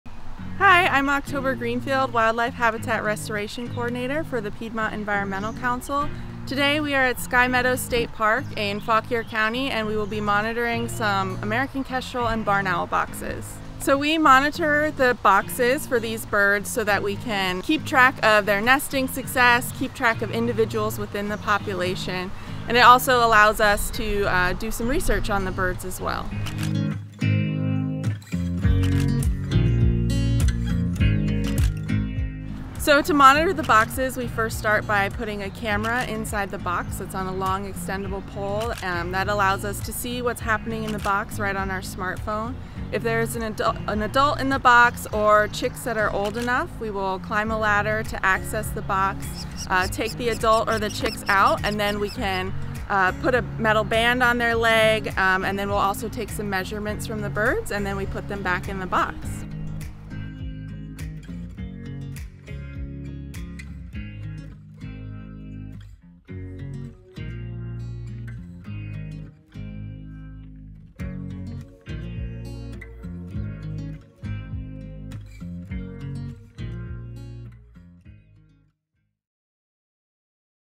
Banding American Kestrels
PEC’s October Greenfield has been busy banding American Kestrels across our region to help track the movement and reproductive patterns of these beautiful falcons.

Want to help kestrels and other grassland birds thrive on your property? to learn more about the Piedmont Grassland Bird Initiative (our partnership with Virginia Working Landscapes, Quail Forever, and American Farmland Trust), or reach out to October directly at !